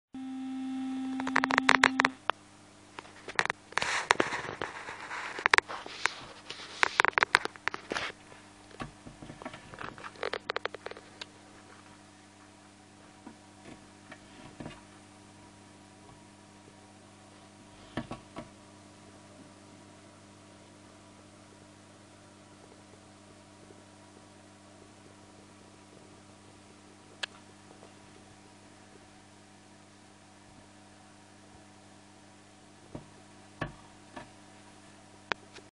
El sapo Sepo y el tritón Marién
2016.
Os presento a mi bombina orientalis y mi paramesotritón honkonguensis (de 10 años)